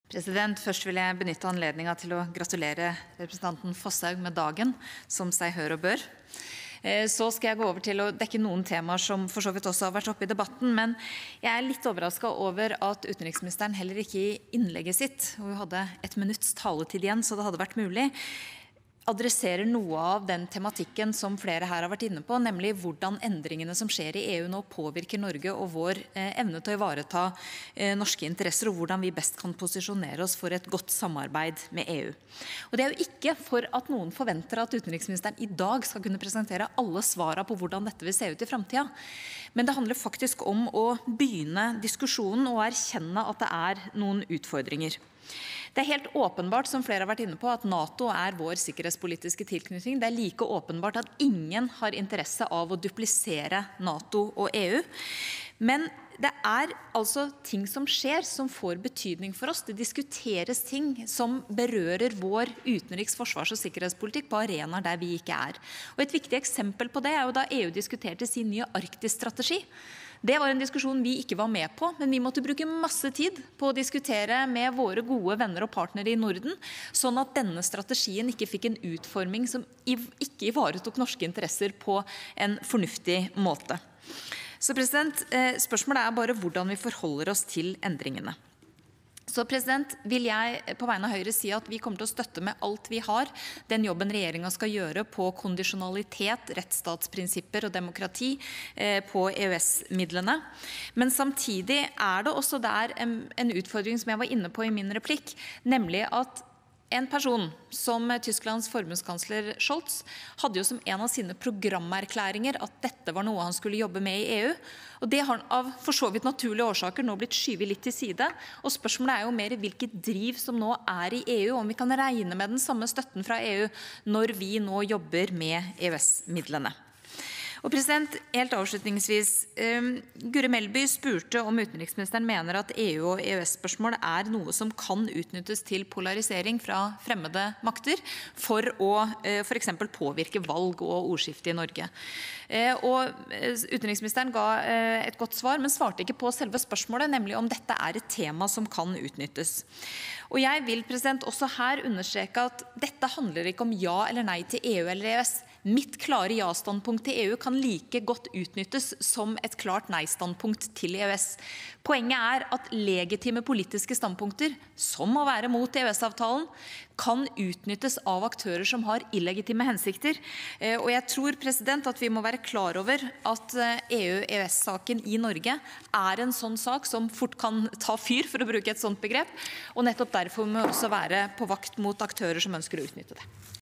EU/EØS-saker | 22.11.2022 Sak 2 7 Ine Eriksen Søreide
Om utenriksministerens redegjørelse om viktige EU- og EØS-saker (Redegjørelsen holdt i Stortingets møte 17. november 2022)